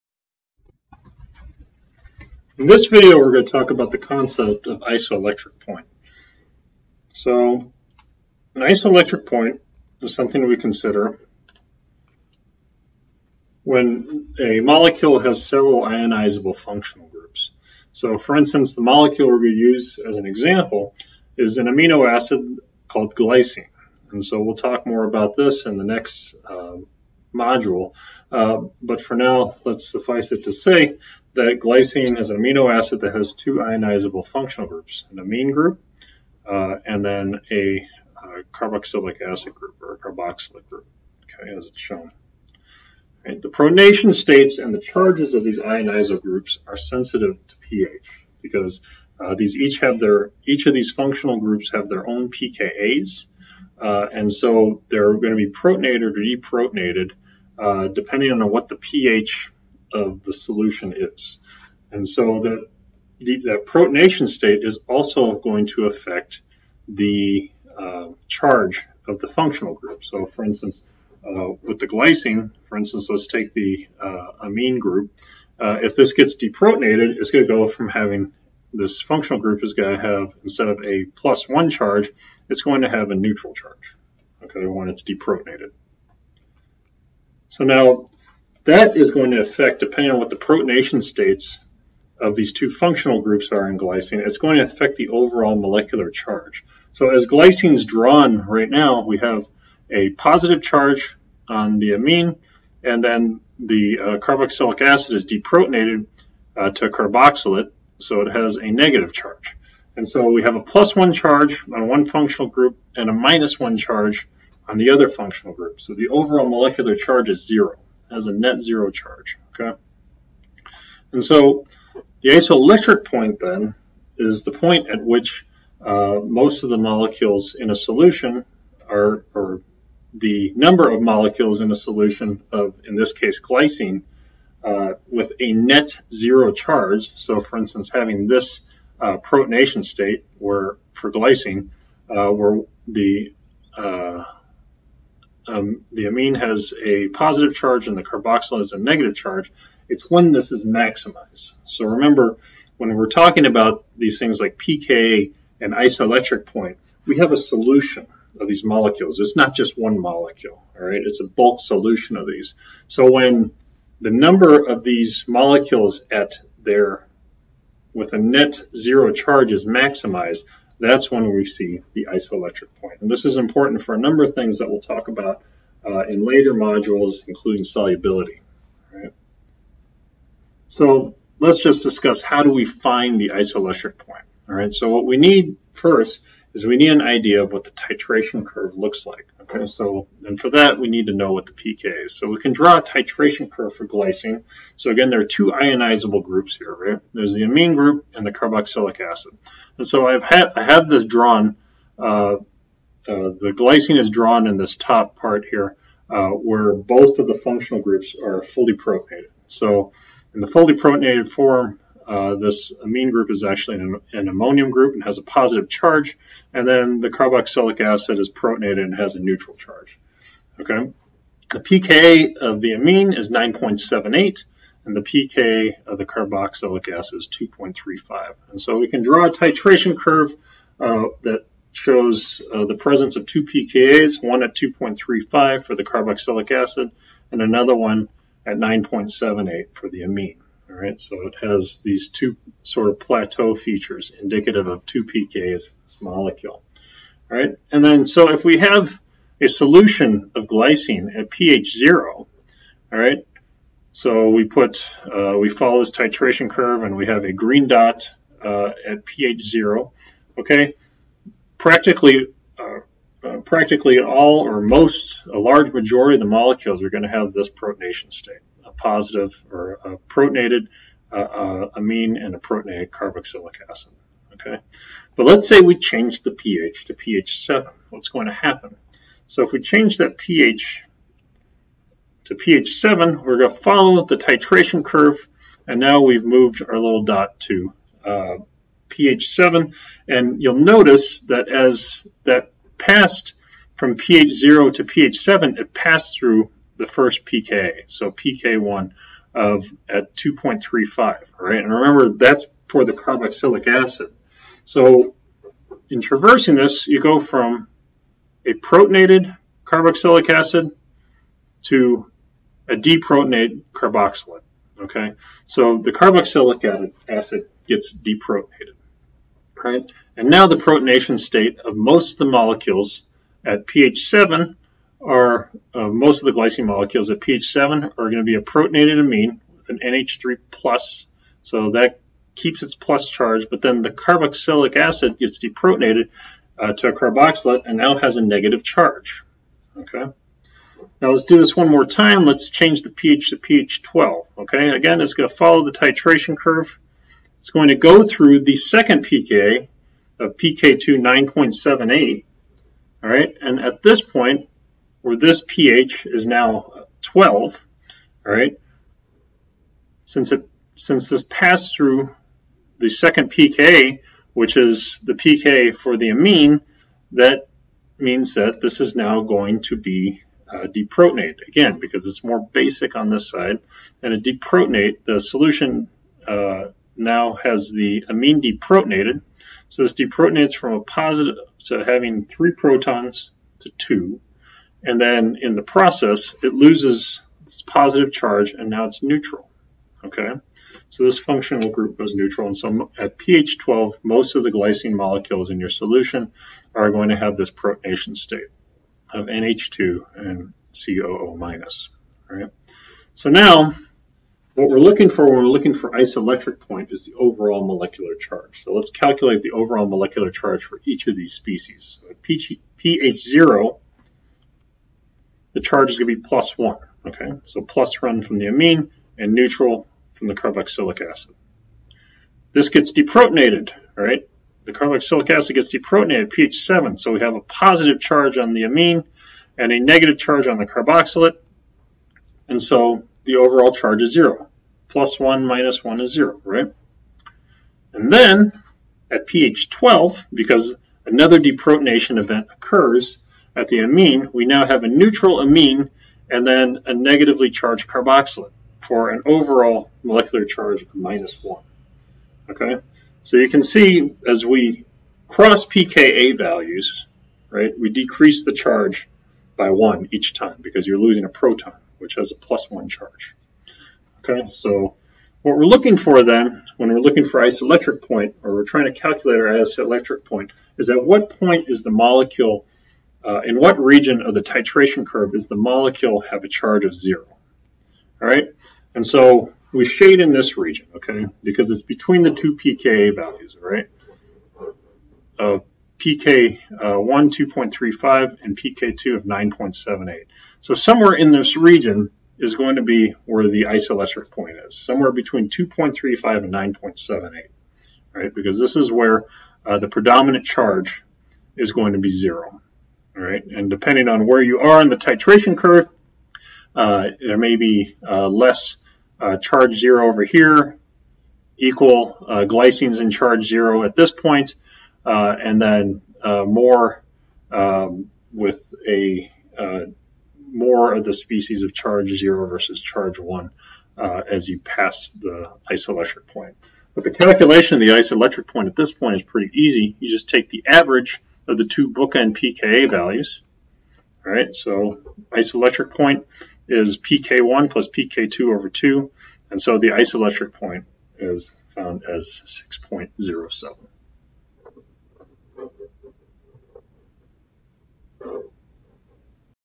Topic 2.11 - Isoelectric point default
Definition of isoelectric point and how to find the isoelectric point of the amino acid glycine.